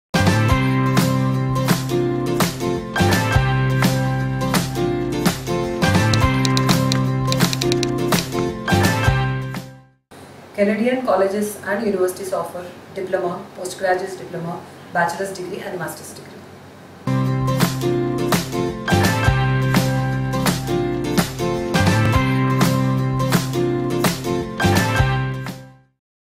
Types of study programs offered in Canada
Take a short note on  types of programs available at universities, university colleges & acadmic institutes in Canada.